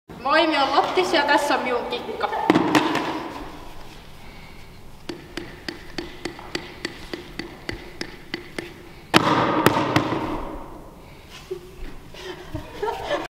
Kikkavideo #5: Tarkkuuspomputusta ämpäriin
Pallon pomputtaminen on tässä kohtaa jo opittu, mutta sen saaminen ämpäriin on omanlaisensa haaste. Mallisuorituksen näyttää Lotta Anttonen.